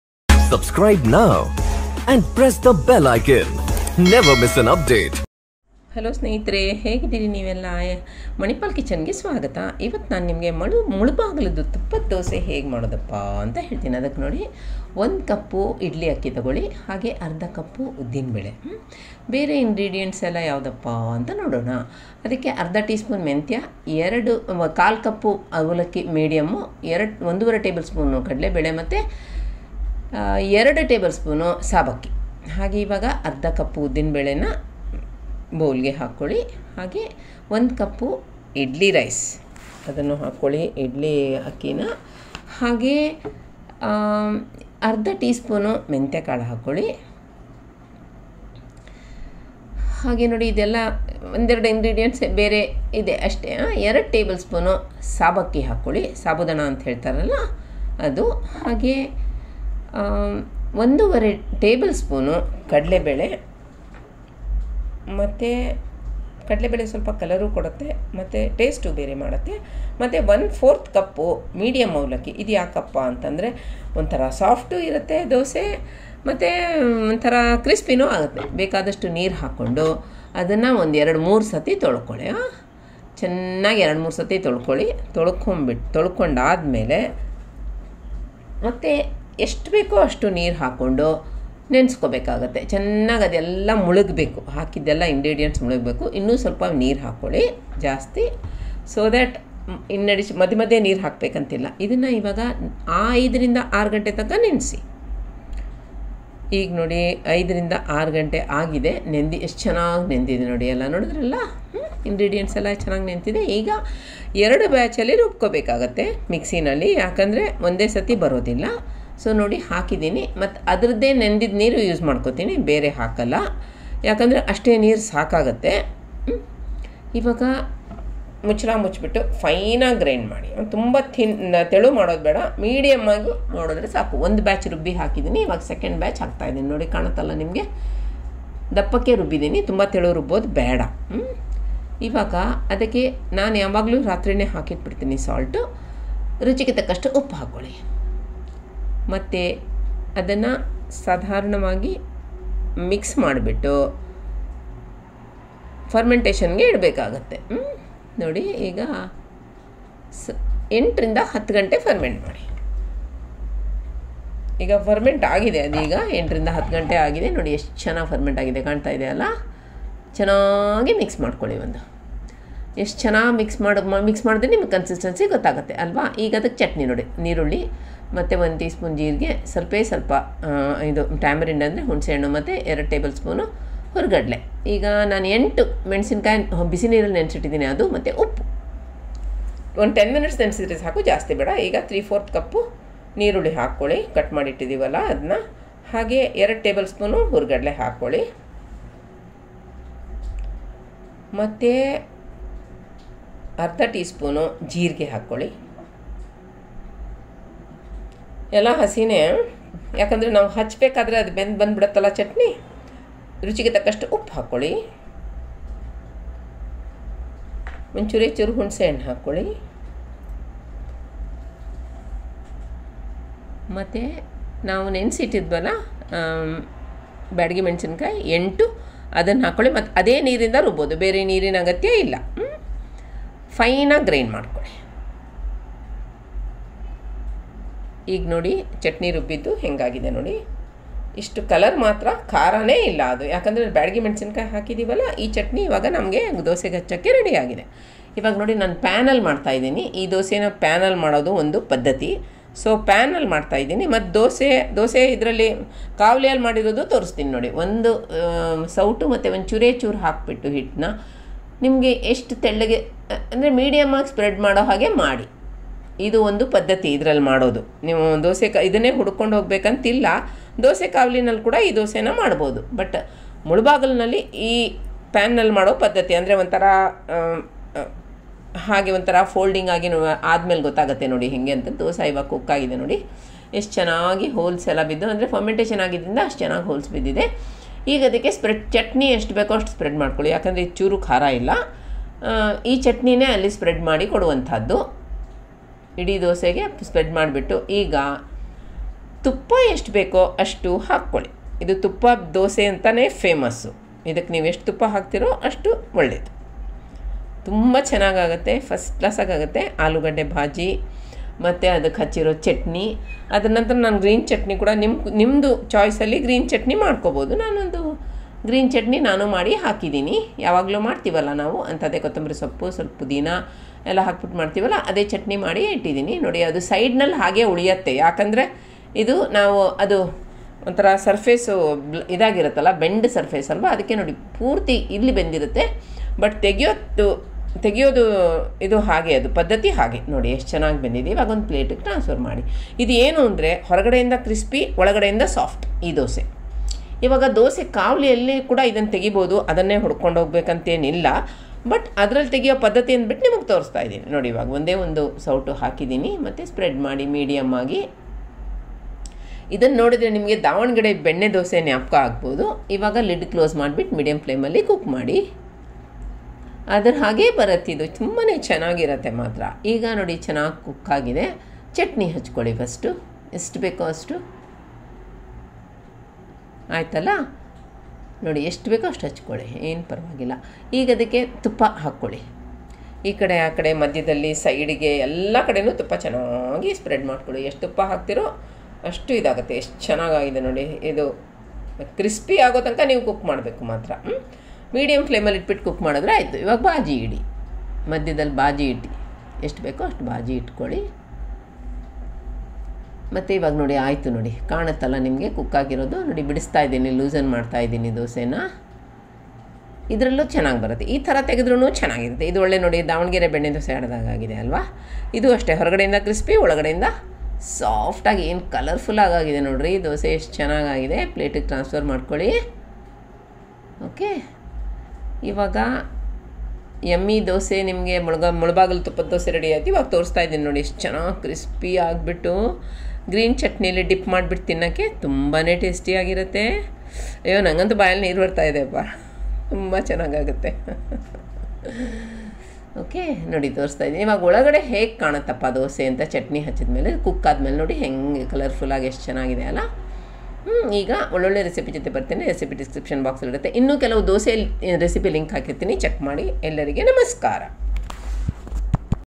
ಮುಳಬಾಗಿಲು ತುಪ್ಪದ ದೋಸೆ | Mulabagilu Tuppada Dose|Manipal Kitchen English Sub-Titles
Mulabagilu Tuppada Dose |ಗರಿಗರಿಯಾದ ,ರುಚಿಯಾದ ಮುಳುಬಾಗಿಲ್ ತುಪ್ಪದ ದೋಸೆ & ಕೆಂಪು ಚಟ್ನಿ | Manipal Kitchen | English Sub-Titles
Mulabagilu is a place in Karnataka and the dosa served there is named after the same as they say Mysore Masala Dosa named after Mysore.  The ingredients of the dosa could be the same with a difference of one or two small ingredients which definitely makes a big difference in the taste and texture of dosa.  Here goes my version of Mulabagilu Tuppada Dose.
INGREDIENTS:
1. Dosa Rice - 1 cup
2. Urad dal - 1/2 cup
3. Methi seeds - 1/2 tsp
4. Medium poha - 1/4 cup
5. Chana dal - 1 & 1/2  tbsp
6. Sabudana pears - 2 tbsp
7. Salt - to taste
8. Ghee - as needed to make dosa
PROCEDURE: Wash  all the ingredients in a bowl and add more water to soak for about 5-6 hours. Grind in a mixer  adding water in steps to a fine consistency adding the same water used for soaking and nothing extra but to a medium thick consistency.  Allow to ferment for about 8-10 hours if it is winter and in summer it can ferment in 5-6 hours adding salt.  Next morning it is fermented well as shown in the video and mixe well till combined and the consistency is just right to make dosas.   Place a dosa griddle on medium flame and apply little ghee and rub it all over the base of the griddle.  Sprinkle water to rub off the extra ghee. Pour a ladle of dosa batter and spread it in a circular motion to as thin as possible to a medium thickness. Close the lid and cook on medium flame.  Spread the chutney and drizzle  more ghee all over and cook till it is crispy and brown.Place potato bhaji in the middle and fold the dosa and transfer to a plate.  Yum Yum!
Makes : around 12- 15 dosas

RED CHUTNEY RECIPE:
iNGREDENTS OF THE RECIPE:
1. Chopped onion - 3/4 cup
2. Hurigadale - 2 tbsp
3. Jeera - 1/2 tsp
4. Salt - as needed
5. Tamarind - a bit
6. Byadgi chili - 8 (soaked in hot water for 10 minutes)
 Grind all the ingredients to a blender bowl and grind it to a fine consistency adding the samne water in which byadi chili was soaked.  Transfer to a bowl and no need to add water as it needs to be thick to apply to the dosa all over .  Yum Yum
Other dosa Recipe links on this channel :
2.